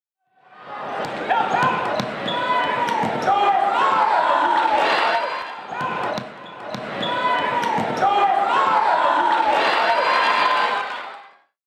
Can't-Miss Highlight: Florence's Danielle Zahn drops a dime
Florence's Danielle Zahn drops a dime to Emma Stensrud for the layup in the Falcons' win over Missoula Loyola.